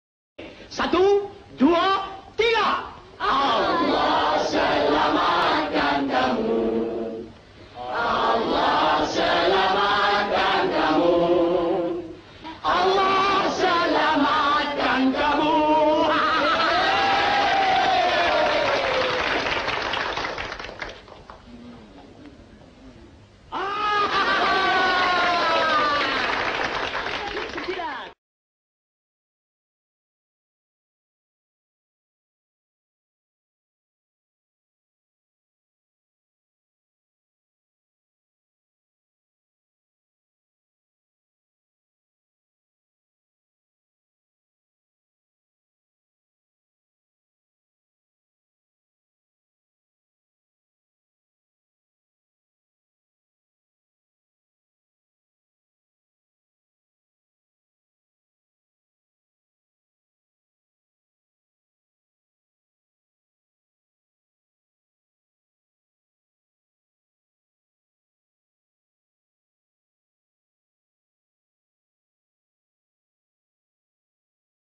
IzaHatta Birthday 2012
Birthday wish by arwah Ahmad Nisfu for Iza Hatta